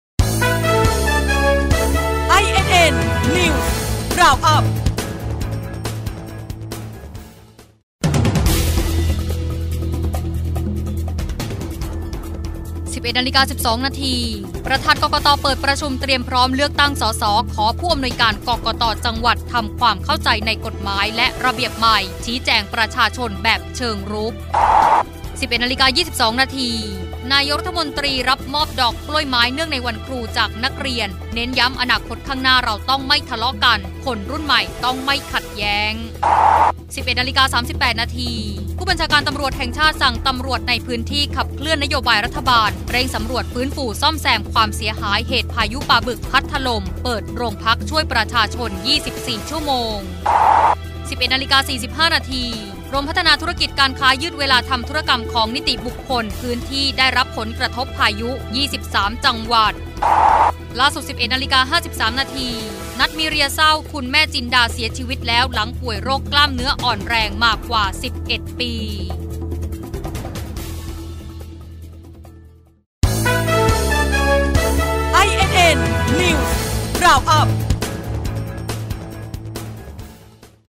เกาะสถานการณ์ 12.30 น.(08-01-62)
11.12 น. ประธาน กกต.เปิดประชุมเตรียมพร้อมเลือกตั้ง ส.ส. ขอ ผอ.กกต.จังหวัดทำความเข้าใจในกฎหมาย-ระเบียบใหม่ ชี้แจงประชาชนแบบเชิงรุก

11.22 น. นายกฯรับมอบดอกกล้วยไม้เนื่องในวันครู จากนักเรียน เน้นย้ำ อนาคตข้างหน้าเราต้องไม่ทะเลาะกัน คนรุ่นใหม่ต้องไม่ขัดแย้งกัน

11.38 น. ผู้บัญชาการตำรวจแห่งชาติ สั่งตำรวจในพื้นที่ขับเคลื่อนนโยบายรัฐบาล เร่งสำรวจฟื้นฟู ซ่อมแซม ความเสียหายเหตุพายุปาบึกพัดถล่ม เปิดโรงพักช่วยปชช.24ชม.

11.45 น. กรมพัฒนาธุรกิจการค้า ยืดเวลาทำธุรกรรมของนิติบุคคล พื้นที่ได้รับผลกระทบพายุ  23 จังหวัด

11.53 น. "นัท มีเรีย" เศร้า คุณแม่จินดา เสียชีวิตแล้ว หลังป่วยโรคกล้ามเนื้ออ่อนแรง มากว่า 11 ปี